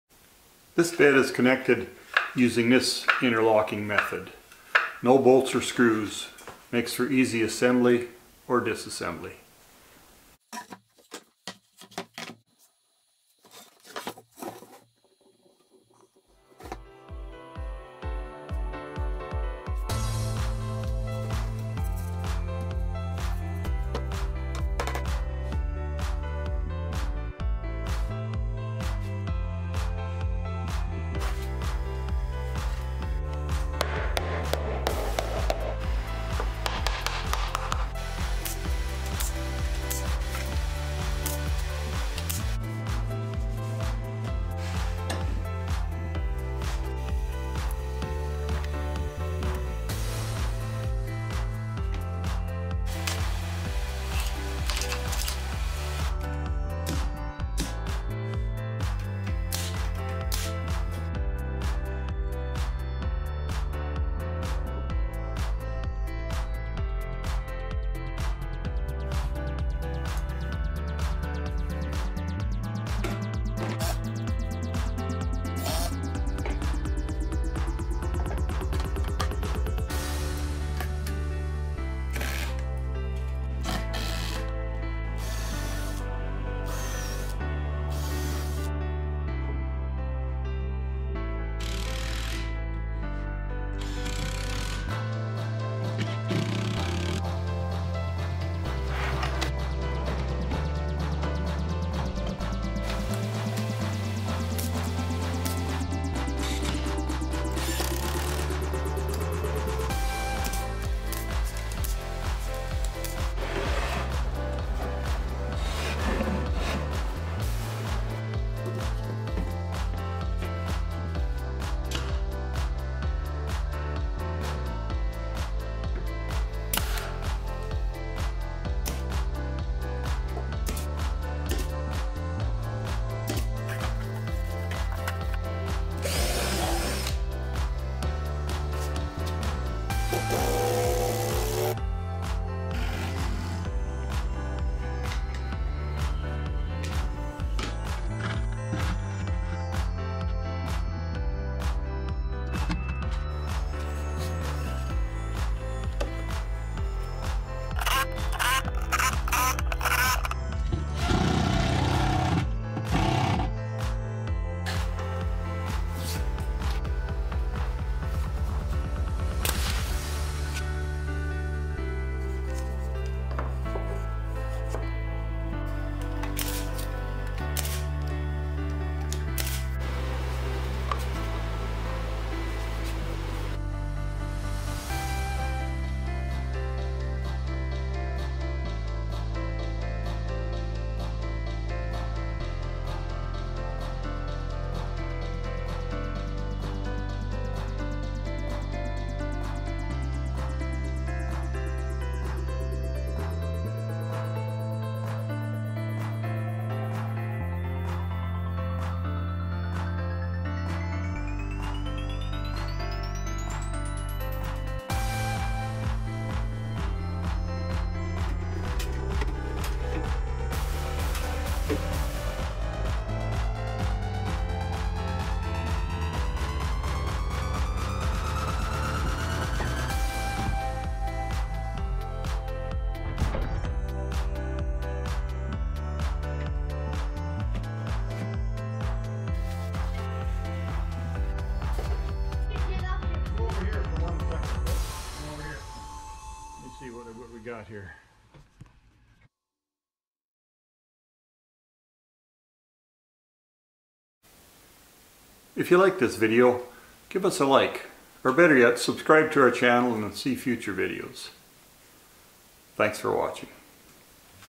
DIY Bed frame with HEADBOARD and STORAGE drawers
This STURDY D.I.Y. bed frame is EASY to assemble and disassemble. NO bolts or screws to REMOVE, see this SIMPLE and  UNIQUE interlocking system.